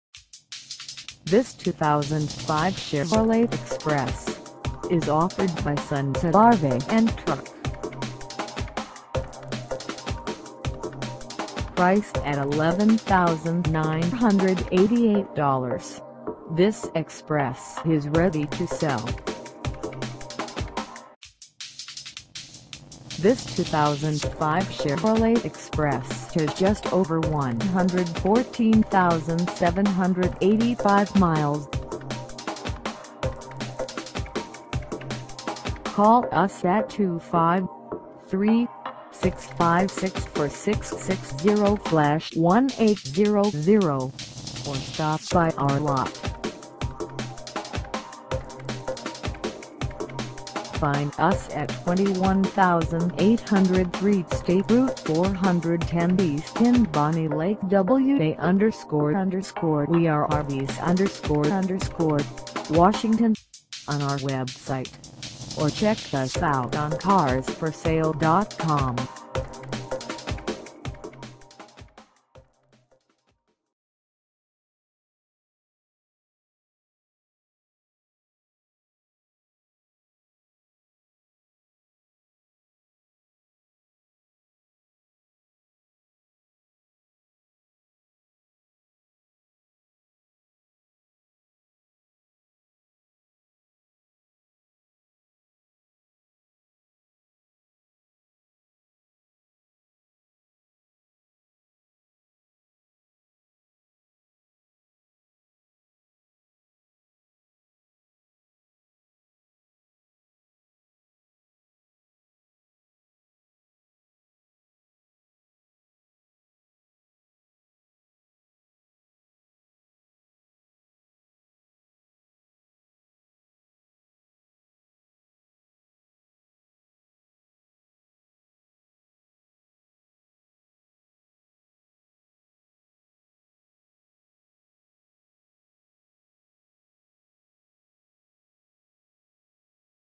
2005 Chevrolet Express 2500 1WT - for sale in Bonney Lake WA __ WE ARE RV'S __ ;, WA 98390
Sunset Rv and Truck
21803 State Route 410 E

in Bonney Lake  WA __ WE ARE RV'S __ ;, WA 98390

Come test dirve this 2005 Chevrolet Express 2500 1WT for sale in Bonney Lake  WA __ WE ARE RV'S __ ;, WA.

EXPRESS yourself at WORK with this 2005 CHEVROLET EXPRESS CARGO VAN with LOW LOW miles!! This 3/4-ton EXPRESS comes with a 6.0L V8, AU transmission, AM/FM/CD player, POWER doors,windows,locks and mirrors.  Call LESA in SUNSET CHEVROLETS COMMERCIAL DEPARTMENT about this BUSINESS/COMMERCIAL VAN and MANY others. Call SUNSET CHEVROLET!!  WE PAY MORE FOR YOUR TRADE! A documentary service fee in an amount up to $150 may be added to the sale price or the capitalized cost.

4-Wheel Disc Brakes,Third Passenger Door,Gasoline Fuel,Daytime Running Lights,Power Steering,Tires - Front All-Season,Tires - Rear All-Season,Intermittent Wipers,A/T,Power Outlet,Engine immobilizer,LQ4 - ENGINE VORTEC 6000 V8 SFI,Brake/transmission shift interlock automatic tran,Rear Wheel Drive,Frame Full-Length Box Ladder Type,Suspension rear hypoid drive axle with multi-le,Steering power,Transmission Overdrive Switch,Brakes 4-wheel antilock 4-wheel disc,Fuel capacity approximate 31 gallon,Exhaust aluminized stainless-steel muffler and ta,Paint solid,Body standard,Daytime Running Lamps,License plate kit front,Wipers intermittent front wet-arm with pulse was,Instrumentation analog includes speedometer odo,Oil life monitor,Warning tones headlamp on key-in-ignition,Theft-deterrent system PASSlock,Defogger side window,Cupholders 3 on engine console cover,Assist handle front passenger,Door Beams Steel-Side,Driver Air Bag,Passenger Air Bag On/off Switch,ABS,Steel Wheels